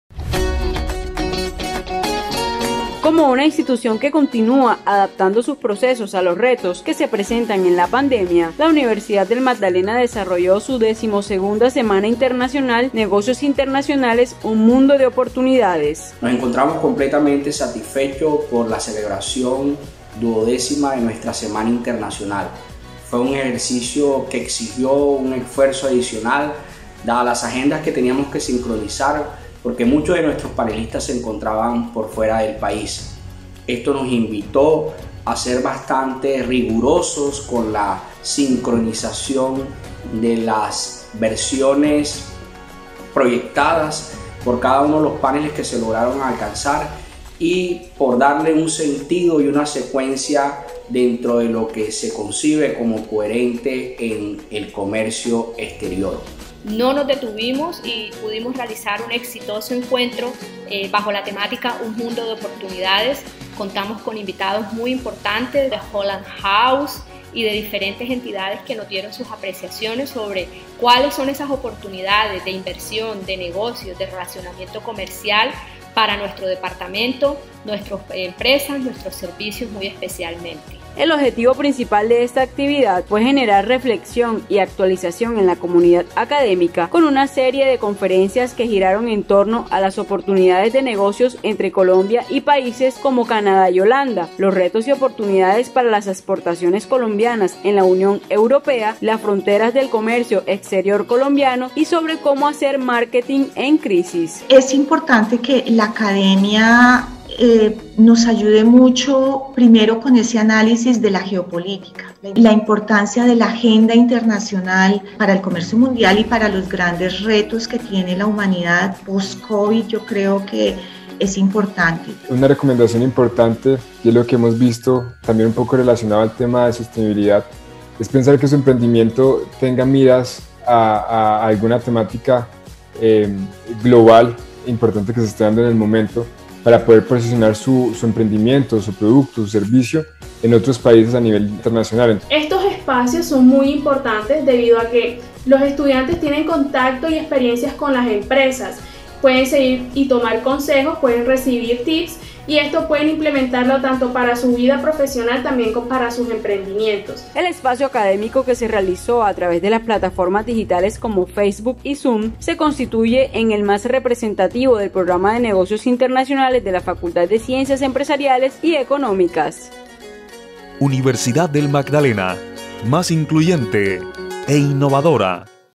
BA276_UNIMAGDALENA DIO A CONOCER ´UN MUNDO DE OPORTUNIDADES´ EN EL COMERCIO INTERNACIONAL
La Alma Mater a través de su Programa de Negocios Internacionales adscrito a la Facultad de Ciencias Empresariales y Económicas, congregó estudiantes, docentes y comunidad en general en las plataformas digitales, para que disfrutaran de dos días de conferencias con invitados nacionales e internacionales que hablaron sobre negocios, comercio internacional, marketing y emprendimiento.